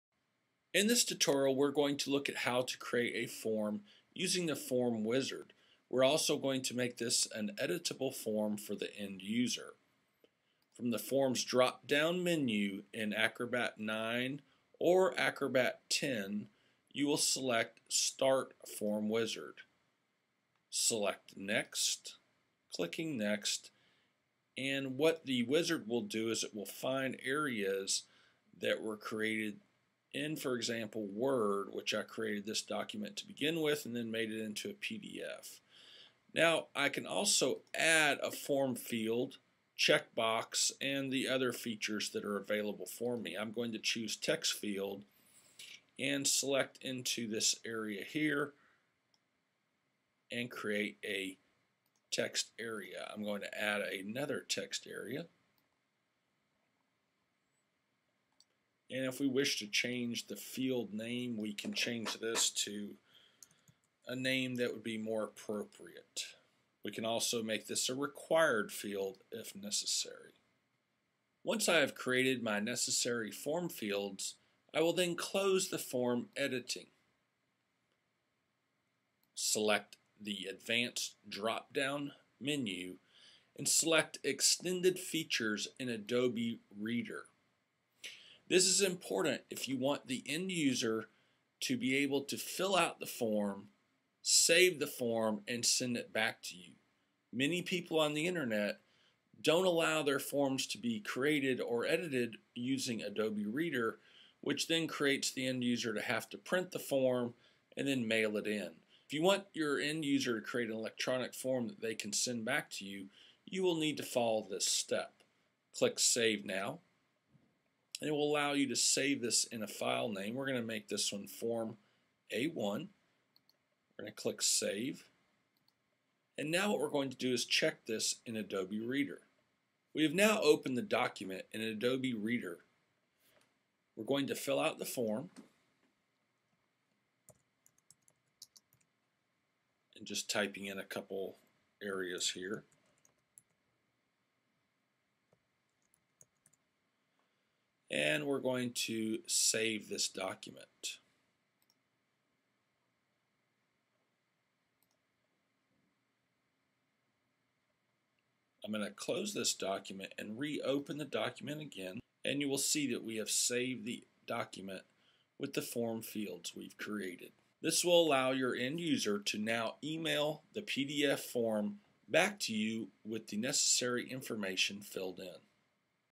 Acrobat - Create Editable Forms
Create editable PDF forms in Acrobat that can be edited in Adobe Reader.